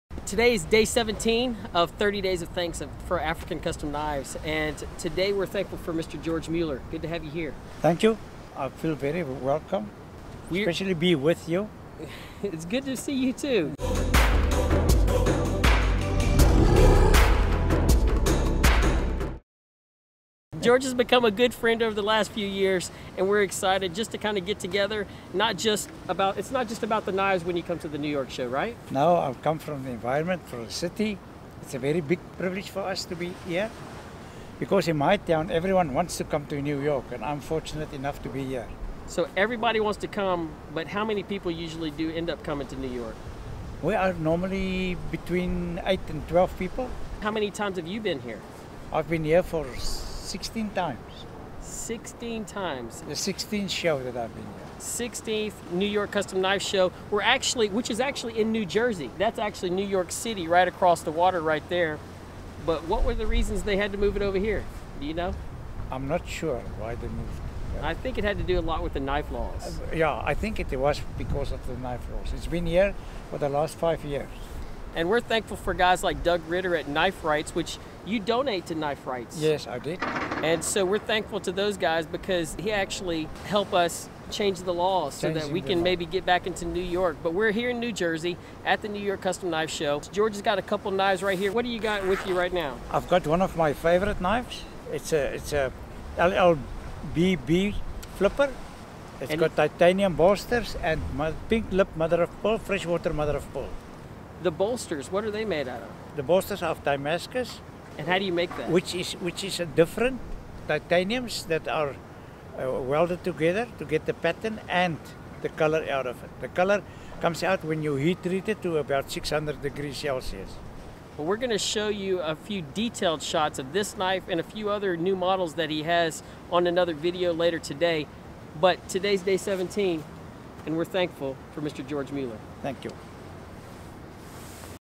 DAY 17 - George Muller - African Custom Knives 30 Days of Thanks
DAY 17 - George Muller - South African Knifemakers
African Custom Knives 30 Days of Thanks

We're celebrating our knifemakers AND our customers this month.

Get awesome deals on incredible knives this month from us at African Custom Knives!